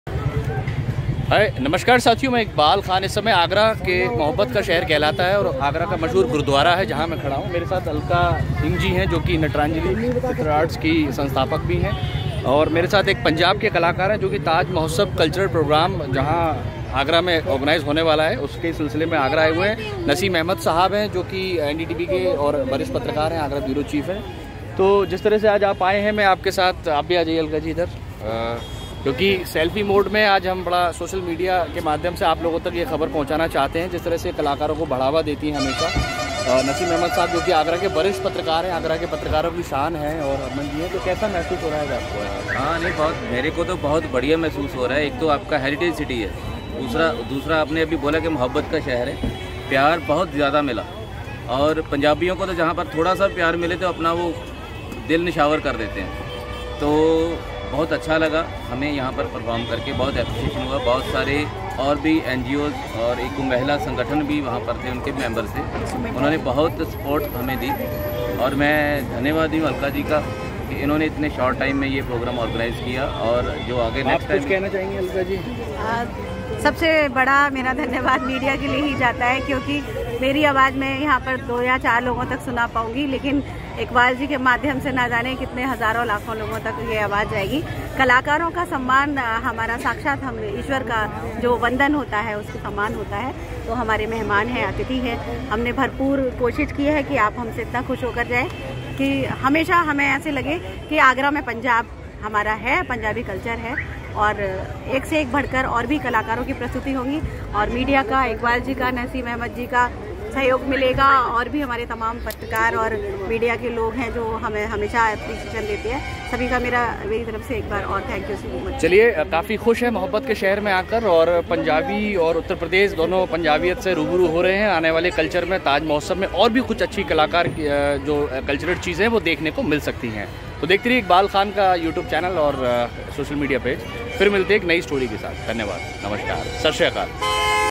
नटरंजली थियेटर आर्ट्स द्वारा आगरा गुरुद्वारे में पंजाब के कलाकारों का किया गया स्वागत#agra
आगरा 27 फरवरी नटरंजली थियेटर आर्ट्स द्वारा गुरदासपुर पंजाब से कलाकारों के दल को रंगारंग कार्यक्रम प्रस्तुति देने के लिए बुलाया गया था इस मौके पर आगरा गुरु के ताल में कलाकारों को सम्मानित किया गया।